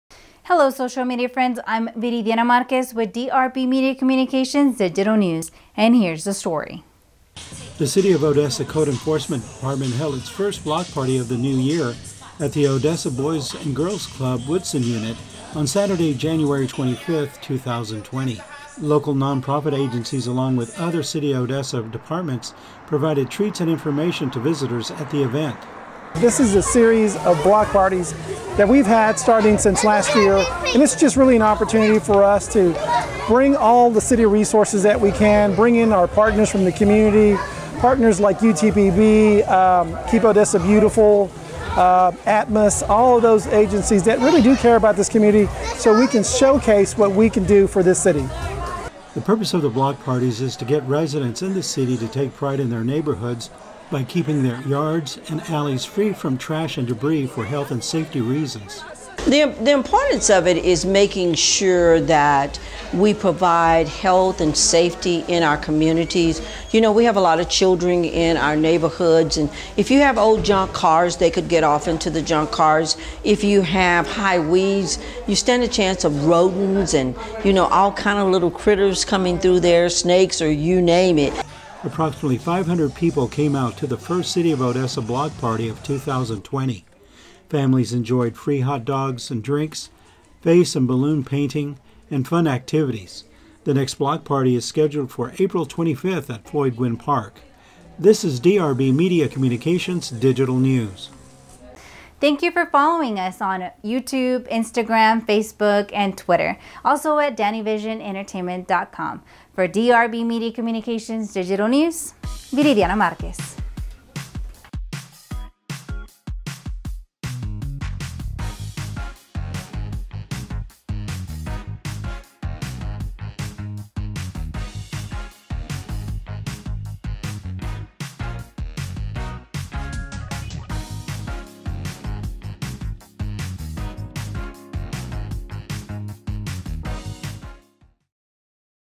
DRB MEDIA COMMUNICATIONS DIGITAL NEWS(1262020)-ODESSA CODE ENFORCEMENT BLOCK PARTY JANUARY 2020
ODESSA, TEXAS/JANUARY 26, 2020-THE CITY OF ODESSA CODE ENFORCEMENT DEPARTMENT HELD IT'S FIRST BLOCK PARTY OF THE NEW YEAR AT THE ODESSA BOYS AND GIRLS CLUB WOODSON UNIT ON SATURDAY, JANUARY 25TH, 2020. LOCAL NON-PROFIT AGENCIES ALONG WITH OTHER CITY OF ODESSA DEPARTMENTS PROVIDED TREATS AND INFORMATION TO VISITORS AT THE EVENT.

(MICHAEL MARRERO, ODESSA CITY MANAGER-ODESSA, TEXAS)

THE PURPOSE OF THE BLOCK PARTIES IS TO GET RESIDENTS IN THE CITY TO TAKE PRIDE IN THEIR NEIGHBORHOODS BY KEEPING THEIR YARDS AND ALLYS FREE FROM TRASH AND DEBRIS FOR HEALTH AND SAFETY REASONS.

(DARLEEN MAYS, CITY OF ODESSA CODE ENFORCEMENT-EDUCATOR)

APPROXIMATELY 500 PEOPLE CAME OUT TO THE FIRST CITY OF ODESSA BLOCK PARTY OF 2020. FAMILIES ENJOYED FREE HOT DOGS AND DRINKS, FACE AND BALLOON PAINTING AND FUN ACTIVITIES . THE NEXT BLOCK PARTY IS SCHEDULED FOR APRIL 25TH, AT FLOYD GWIN PARK.